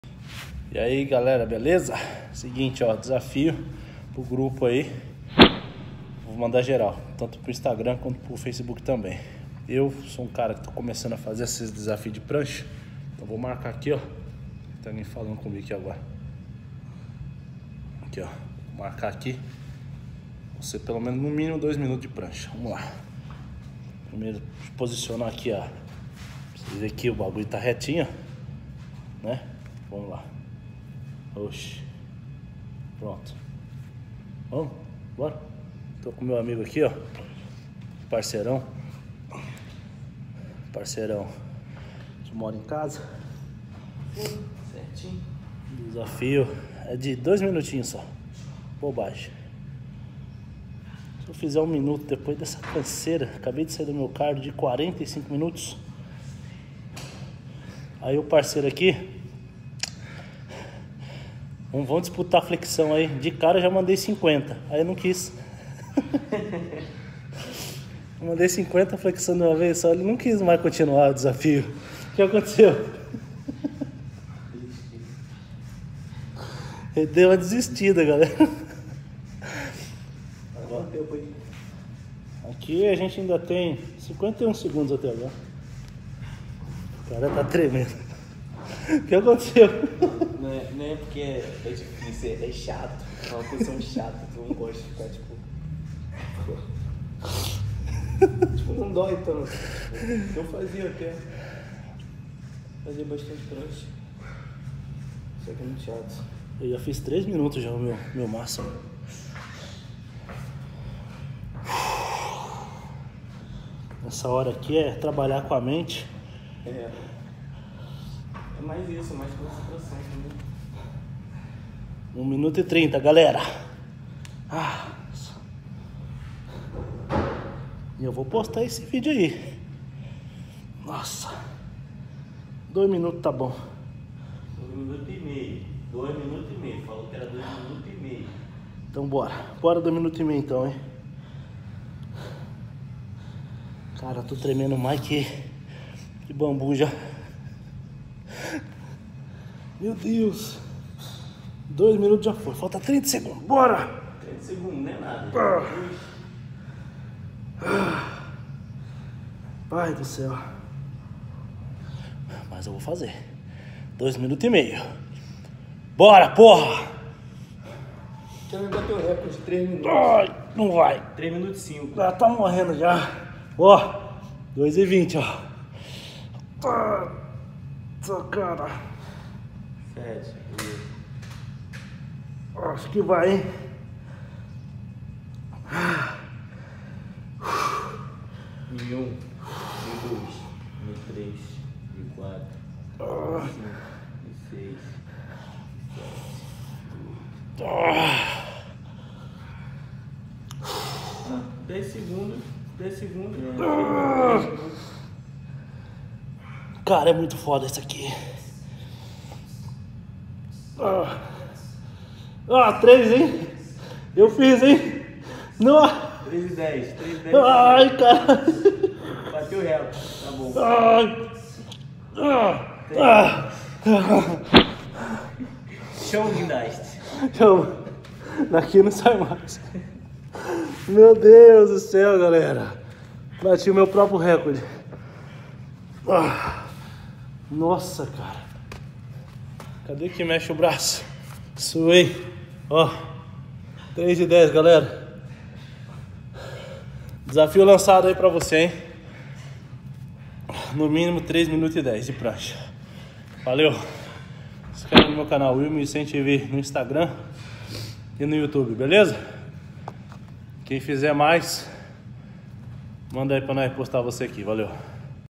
Desafio da prancha
Bora se desafiar ?

Desafio bem simples de prancha para dar aquela fortalecida no abdômen

Quero ver você fazer mais de 3 minutos ...

Siga no Instagram - will1100tv